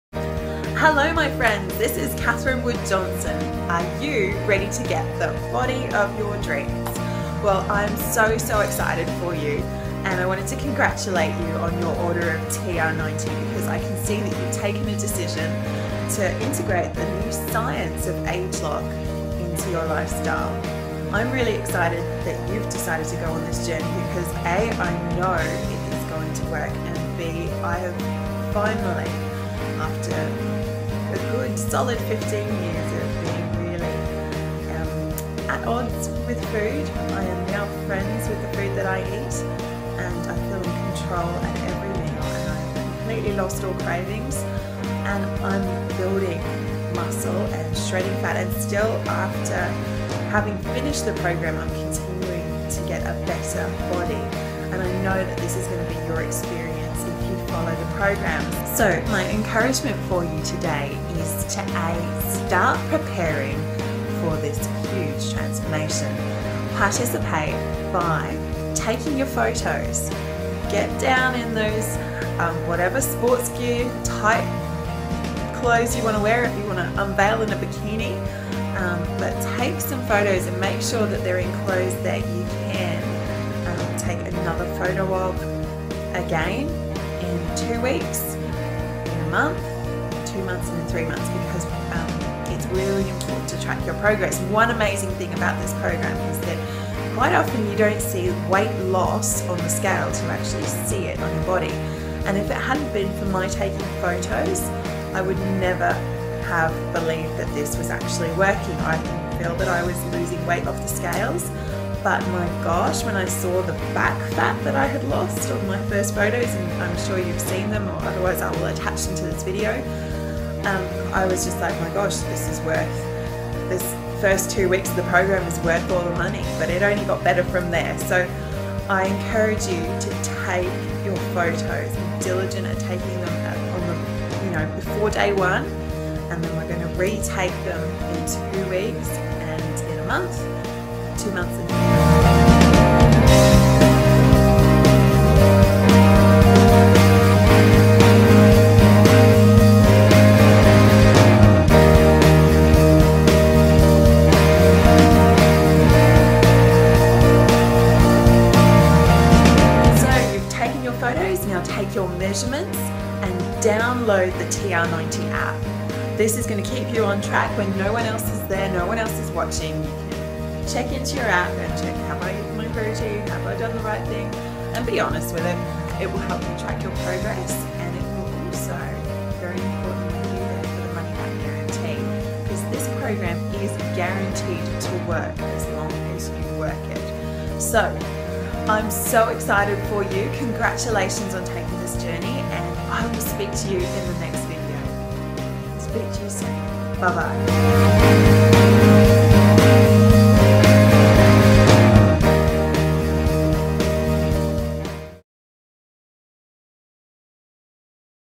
Congratulations on taking the TR90 challenge!
Tr90 isn't a diet, it is a science. The researchers and phd scientists have done their part in formulating this product - your part is to make sure you do your best to follow the eating program and get a minimum of 30minutes of resistance exercise 3 times per week!

Follow the program and get the results you want :-)
Make sure you download the TR90 app or fill it out online with your photos and your measurements!

Here's to your success.
Kindest regards,
Catherine Wood-Johnson